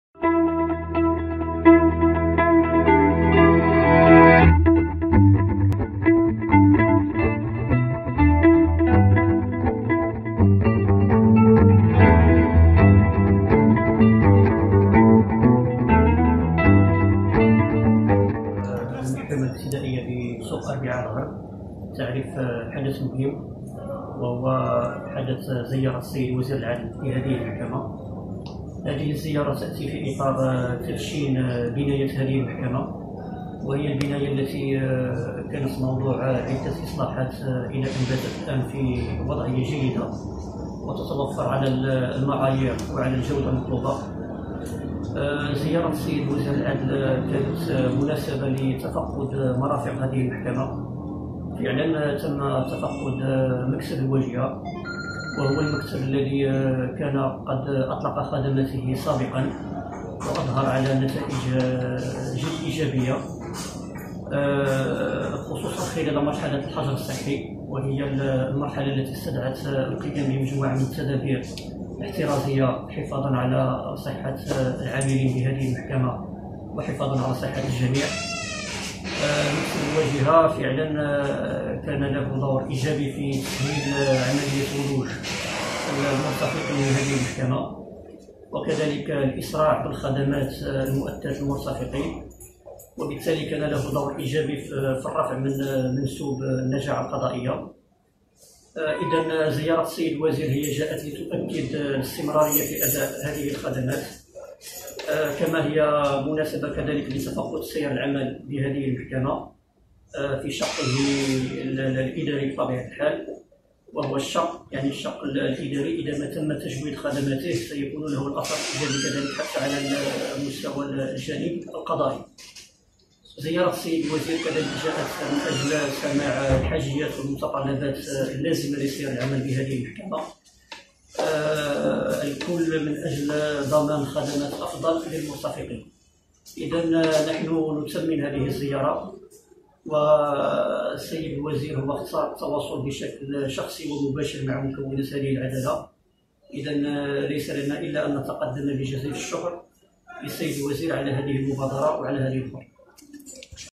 كلمة الأستاذ طارق الحسناوي رئيس المحكمة الابتدائية بسوق أربعاء الغرب على إثر زيارة وزير العدل
كلمة  الأستاذ طارق الحسناوي رئيس المحكمة الابتدائية بسوق أربعاء الغرب على إثر الزيارة الرسمية للسيد  وزير العدل